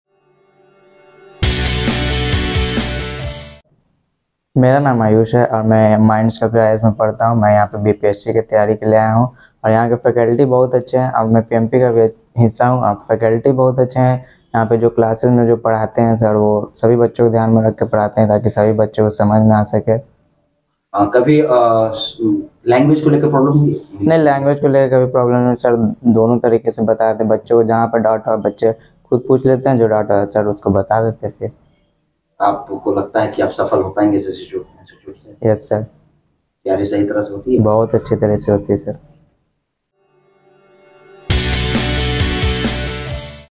Student's Review: Ayush Kumar
@MindSculptorsIAS  brings to the aspirants a series of video showcasing the feedback given by the students about the programs run by the institutions.

Mr. Kumar talks about the way faculties teach in the classroom, how the language barriers are not a problem for students as the faculties concerned take care of their doubts in the class itself.